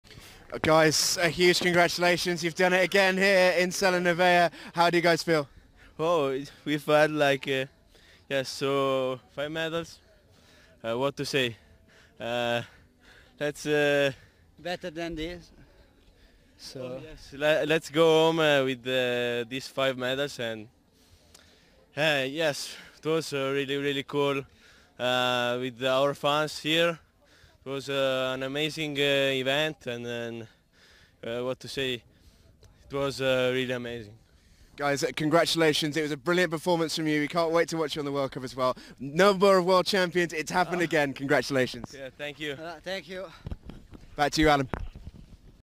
Giacomo Bertagnolli and guide Fabrizio Casal Race Reaction | Super Combined | 2019 WPAS Champs
Race reaction and interview with the men's super combined vision impaired world champions, Giacomo Bertagnolli and guide Fabrizio Casal (Italy) at the 2019 World Para Alpine Skiing Championships in Sella Nevea, Italy.